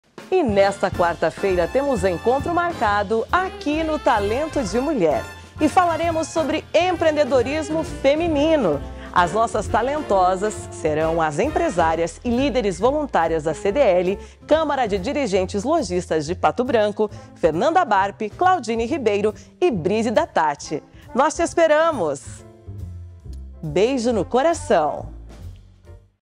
Prévia do Talento de Mulher: Empresárias e líderes da Câmara de Dirigentes Lojistas de Pato Branco
E no Talento de Mulher dessa semana falaremos sobre empreendedorismo feminino. As nossas talentosas serão as empresárias e líderes voluntárias da CDL – Câmara de Dirigentes Lojistas de Pato Branco, Fernanda Barp, Claudine Ribeiro e Brizida Tatsch. Nós te esperamos, beijo no coração!